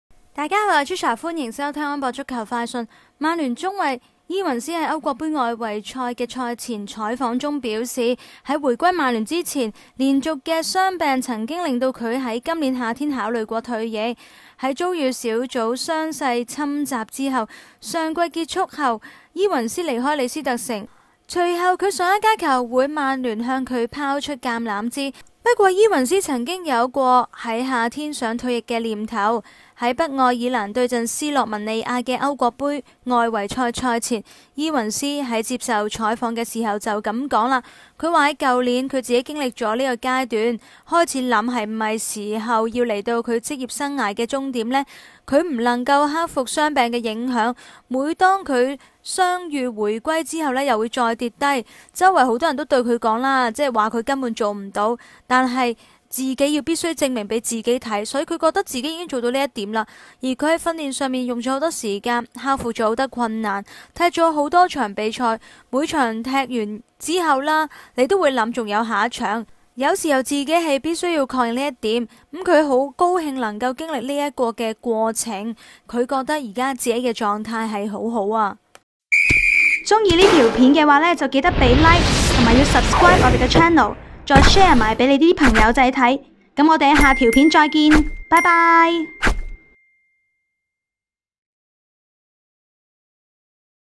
【英超消息】2023-10-18 伊雲斯曾考慮退役 [聲音報導: Trisha]
英超 曼聯 中衛莊尼.伊雲斯 在歐國盃外圍賽的賽前採訪中表示，在回歸曼聯前，連續的傷病曾讓自己在今年夏天考慮過退役。

在遭遇小腿傷勢侵襲後，上賽季結束後莊尼.伊雲斯離開李斯特城，隨後老東家曼聯向他拋出了橄欖枝，不過莊尼.伊雲斯也曾有過在夏天退役的念頭。在北愛爾蘭對陣斯洛文尼亞的歐國盃外圍賽賽前，莊尼.伊雲斯在接受採訪時說：

「我想去年我經歷了這樣一個階段，開始思考「是不是我的職業生涯將要來到終點。」我不能夠克服傷病的影響，每次當我傷癒回歸後又再一次倒下。周圍有很多人對我說「你根本不可能做到」，但你必須自己證明給自己看，我覺得我已經做到了這一點。」

「我在訓練上花費了很多時間，克服了很多困難，踢完了很多比賽。每次踢完一場球你就會想「還會有下一場」。有時候你必須向自己確認這一點，我很高興我能夠經歷這個過程。我覺得我現在狀態很好。」